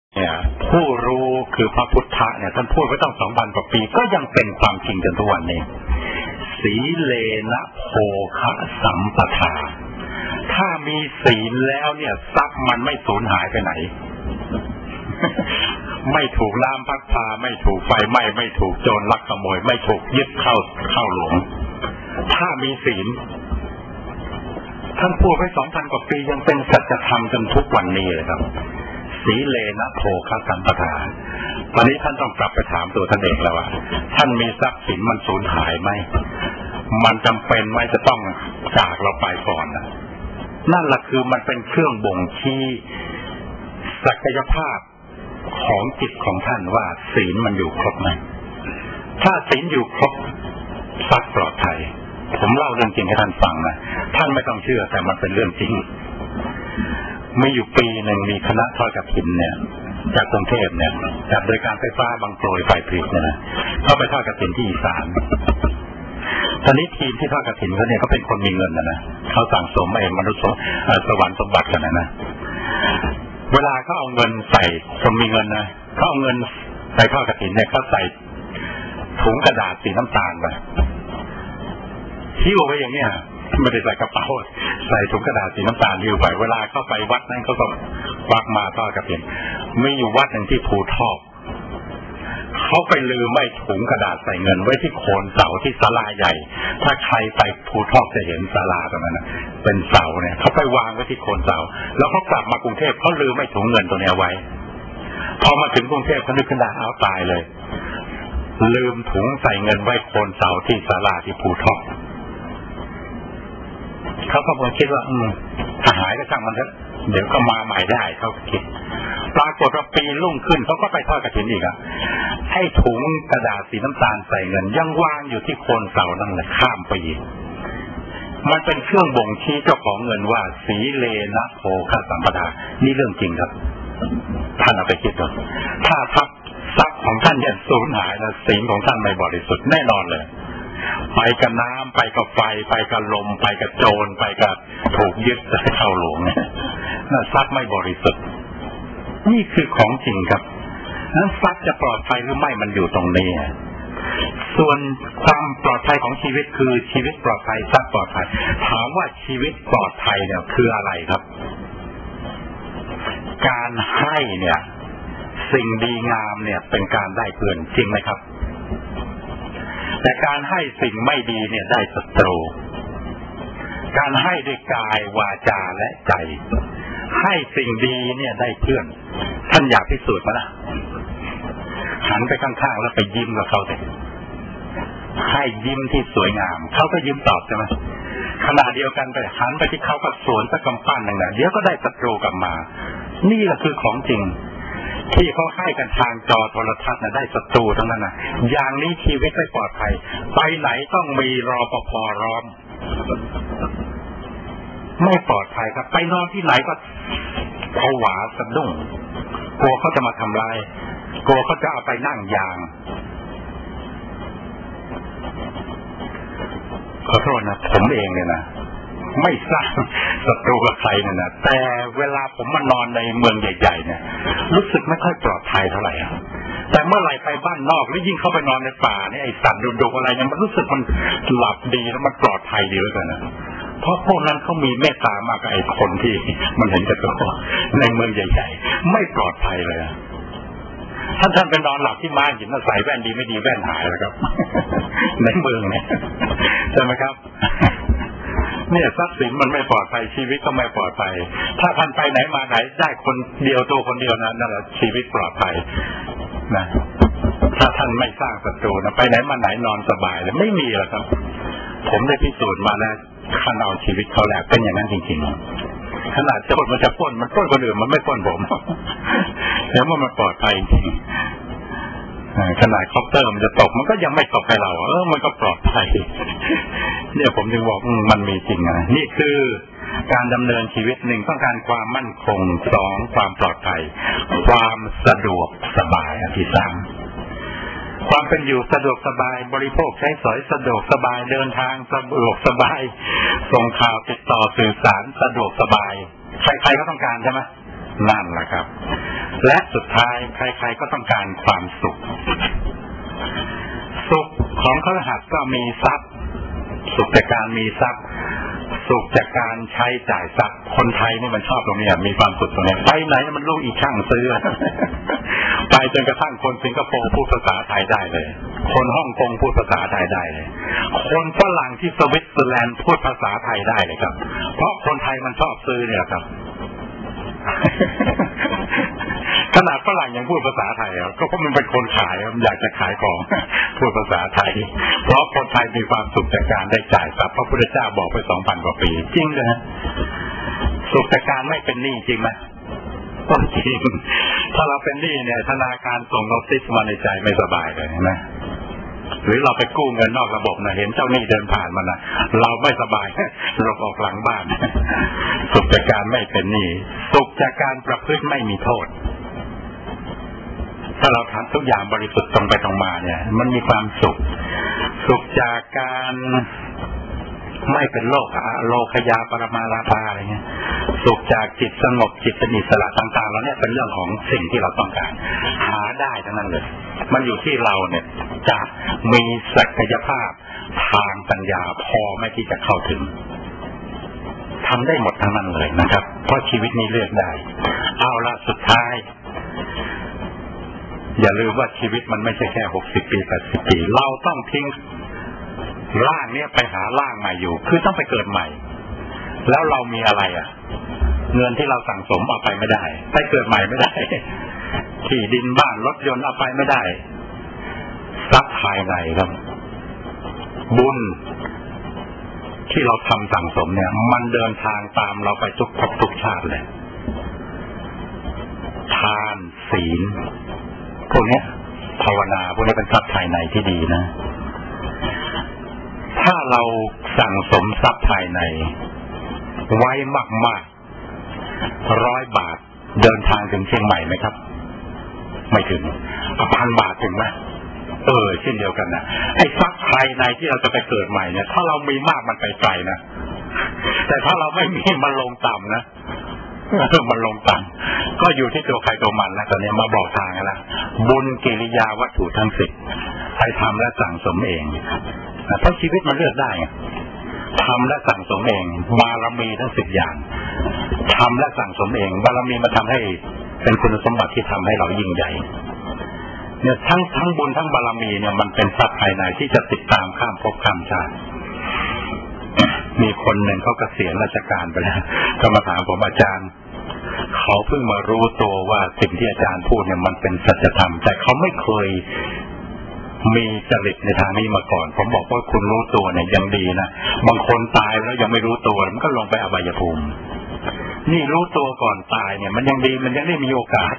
หลักธรรมในการดำเนินชีวิต 14/15
อาจารย์ ดร. สนอง วรอุไร
บรรยายที่กรมศุลกากร วันพุธที่ 5 เมษายน 2549
เรื่อง " หลักธรรมในการดำเนินชีวิตและการปฎิบัติงาน"